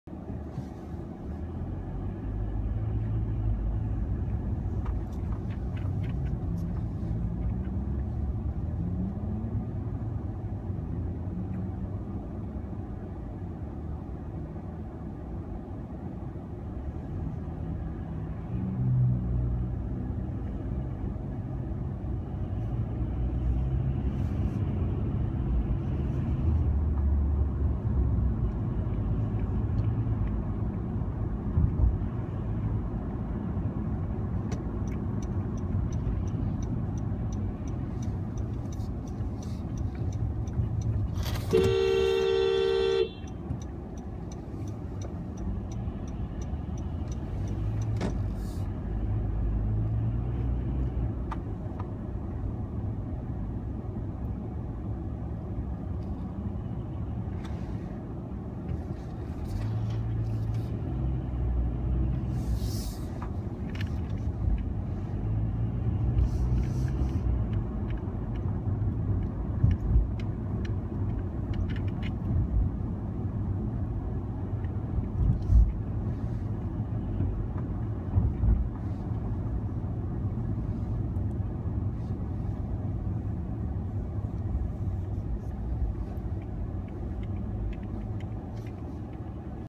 COA attempts & fails again with a right turn duck-and-pit maneuver, using a jalopy half-ton truck
video clip also features another zombie pedestrian wandering in the street... please check out my twitter feed @thcsoy